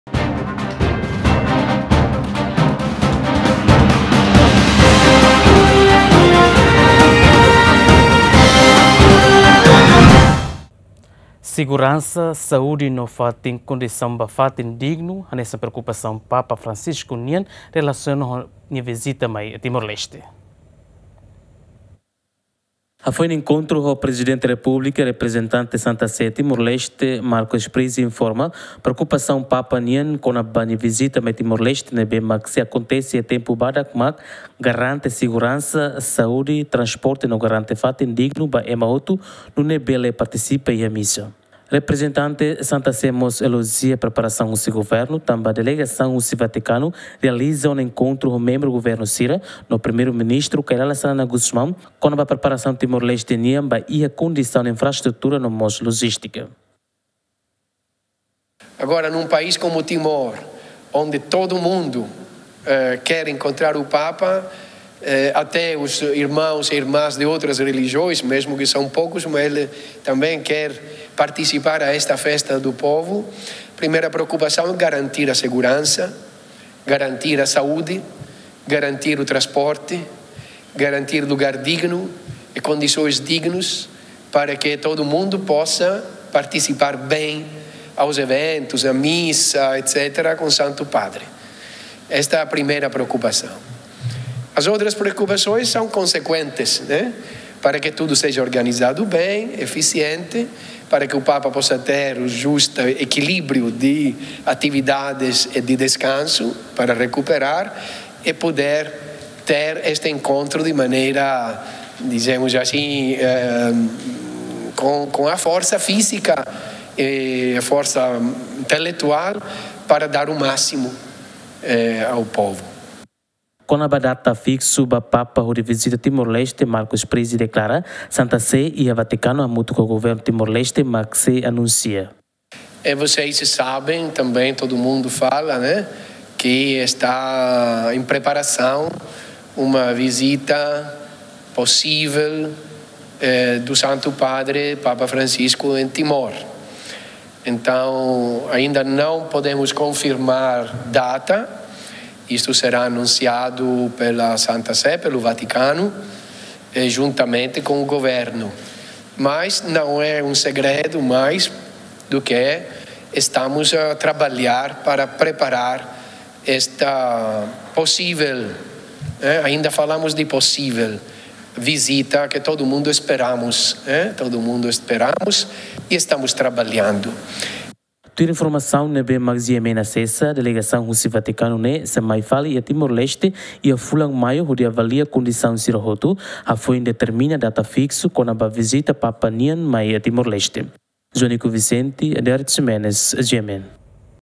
Seguransa Saúde no kondisaun ba fatin dignu hanesan preokupasaun PAPA nian atu vizita mai TL
Seguransa Saúde no kondisaun ba fatin dignu hanesan preokupasaun PAPA Francisco nian relasiona ho nia vizita mai Timor-Leste